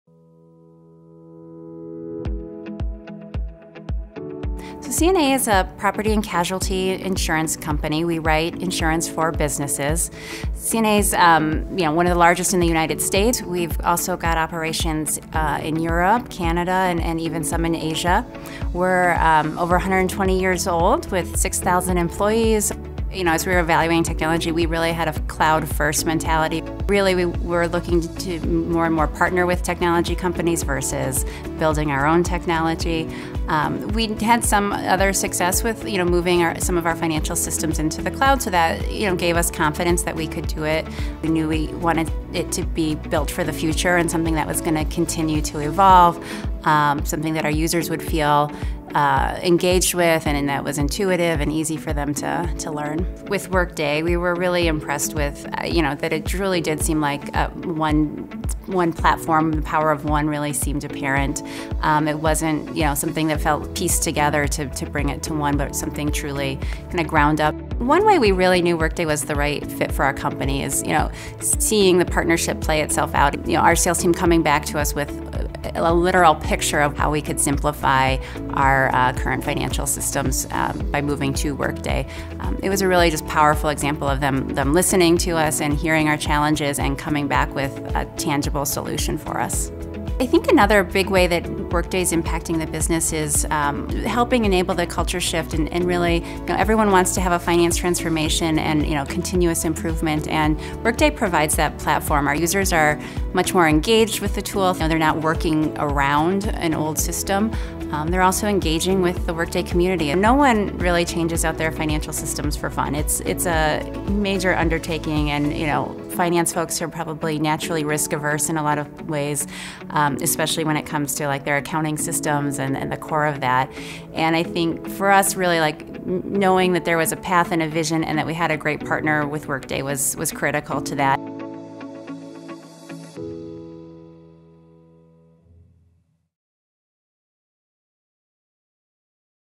Workday and CNA
CNA, headquartered in Chicago, Illinois, was founded more than a century ago and its mission and vision is to focus on what it does best: providing insurance solutions that allow its customers to better manage their risks and grow profitably. Amy Smith, vice president of Accounting and Reporting, explains why CNA selected Workday and the value realized since going live.